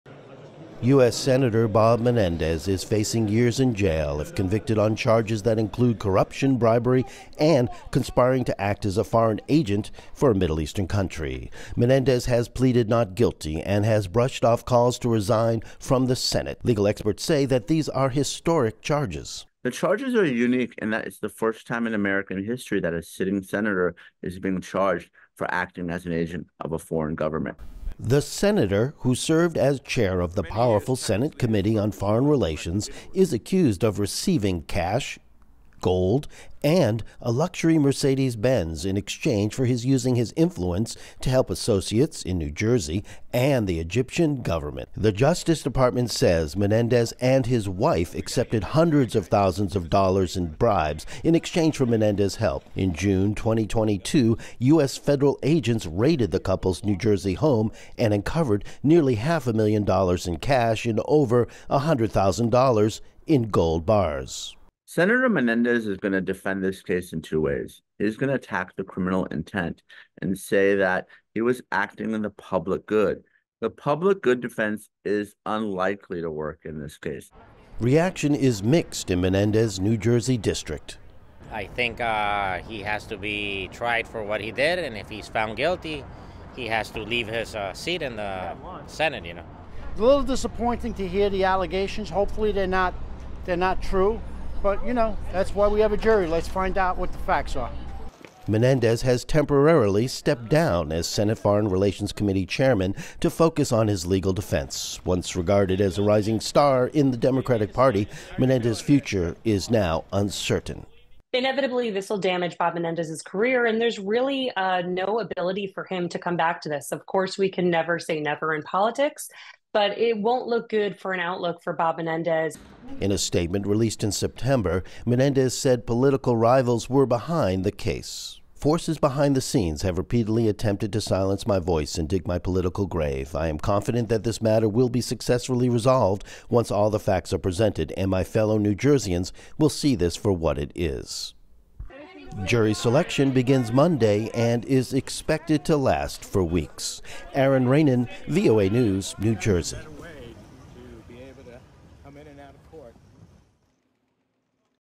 US Senator Bob Menendez set to face federal corruption trial
U.S. Senator Bob Menendez – the former head of the powerful Senate Foreign Relations Committee – is set to go on trial next week for allegedly using his position for personal gain. Jury selection in the federal corruption case is expected to begin in New York City on Monday, May 13. Aron Ranen has more from New York.